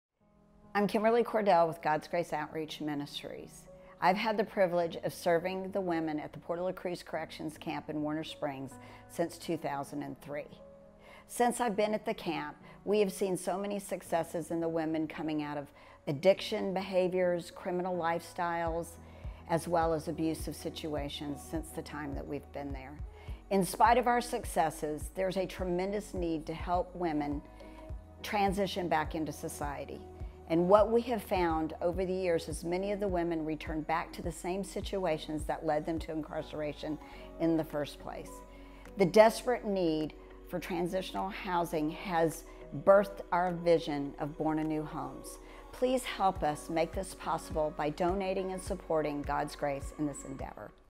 Born Anew Homes for Women Rev. Kimberly Kordell
Born Anew for Women is a voluntary program for women in Southern California transitioning from incarceration back into society. Help us create safe spaces for women to grow into their future of living a healthy life.